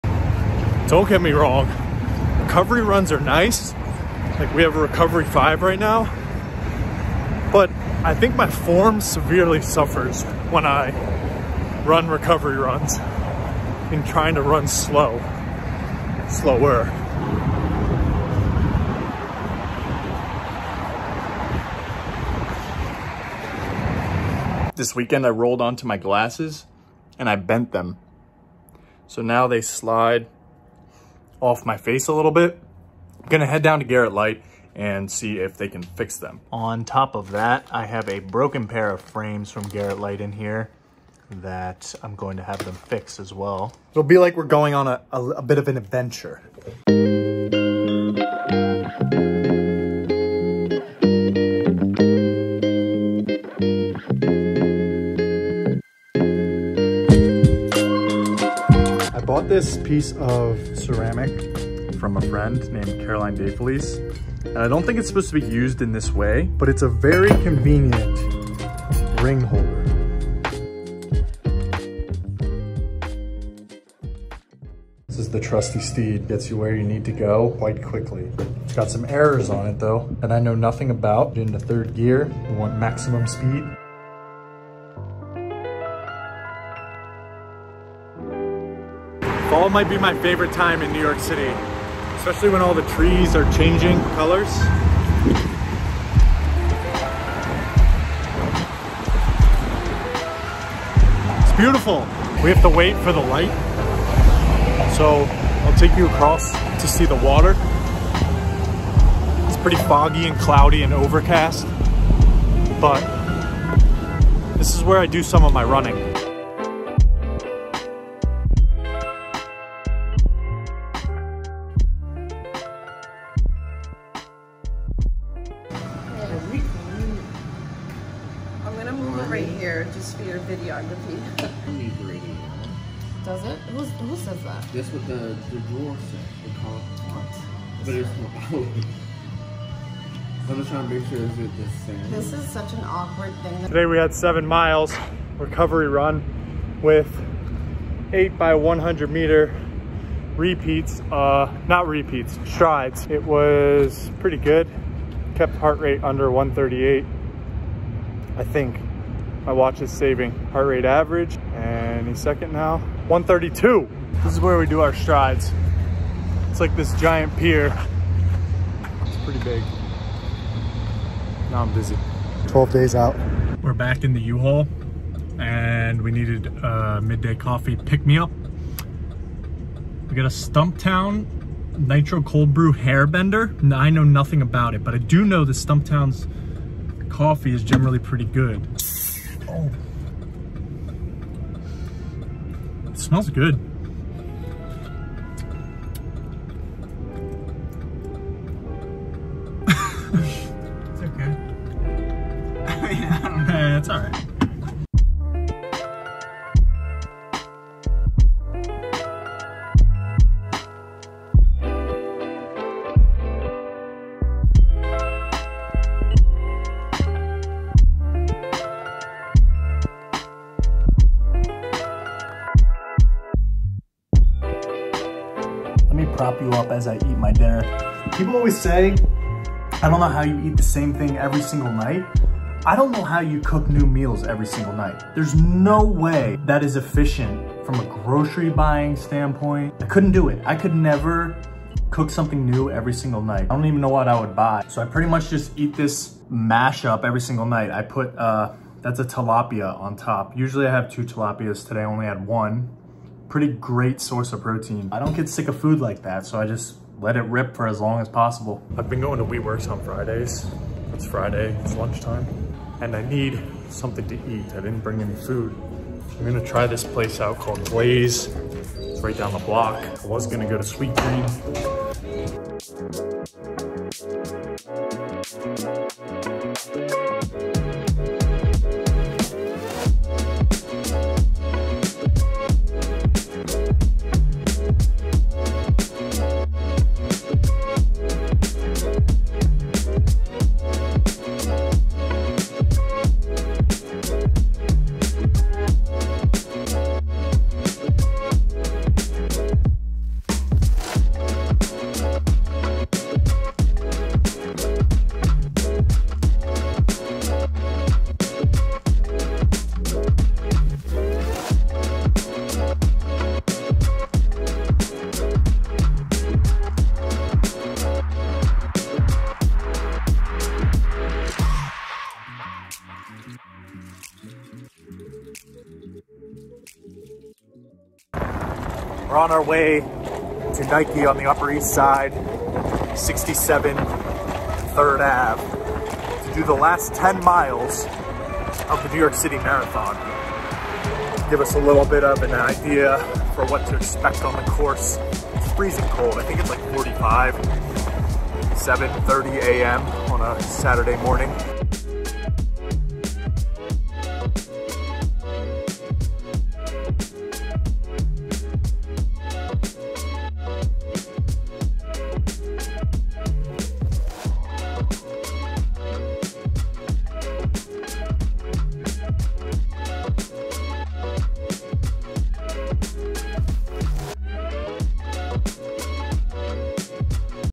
NYC Marathon Taper | 7 Days Out | Vlog
17 weeks of training in the books.  The NYC Marathon will be my first marathon and one I am extremely excited to race.  Training for a marathon has taught me a lot about both the body and the mind.  Here to good weather on race day!